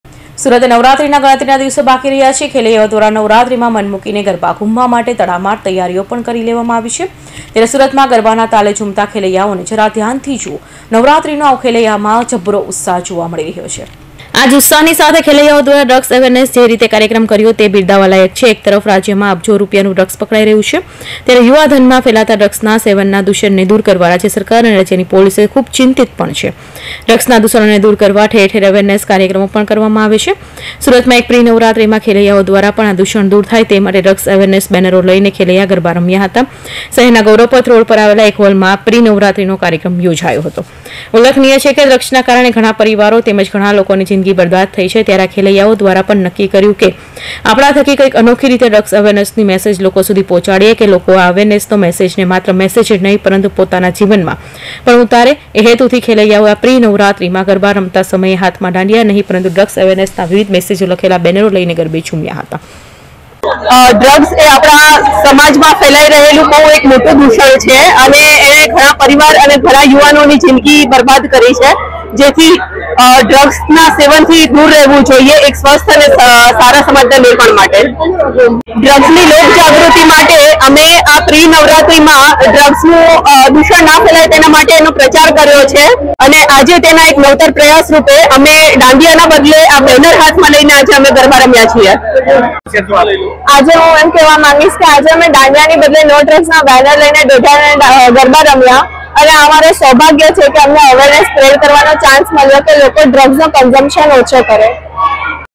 સુરતીઓની અનોખી પ્રિ નવરાત્રી,
હાલ નવરાત્રી પ્રિ નવરાત્રી ઠેર ઠેર ચાલી રહી છે
ડ્રગ્સ અવેરનેશ માટે સુરતીઓ નો અનોખો પ્રયાસ
ડ્રગ્સ અને વ્યસનથી દૂર રહેવાના સંદેશ સાથે ગરબે ઘૂમ્યા ખેલૈયાઓ